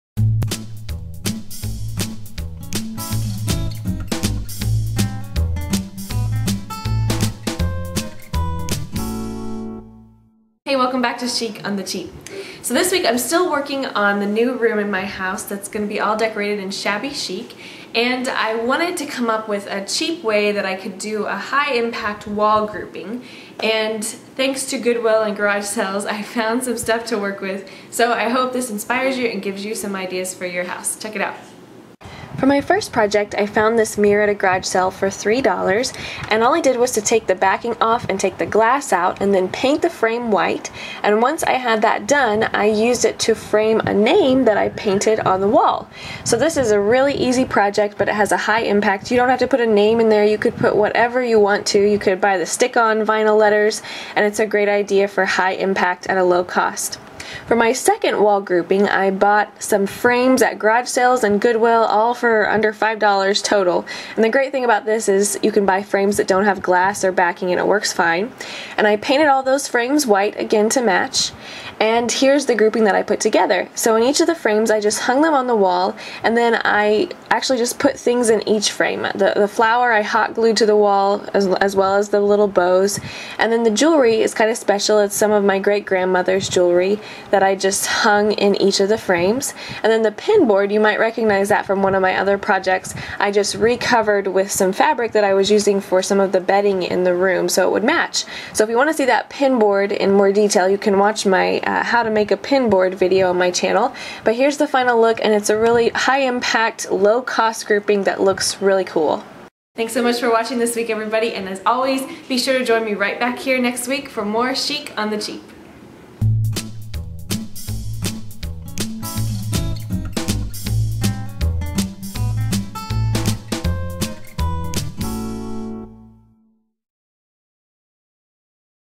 Chic on the Cheap: Wall Grouping Idea
Want a cheap way to compose an eye-catching wall grouping? Watch this quick video and make a masterpiece of your own!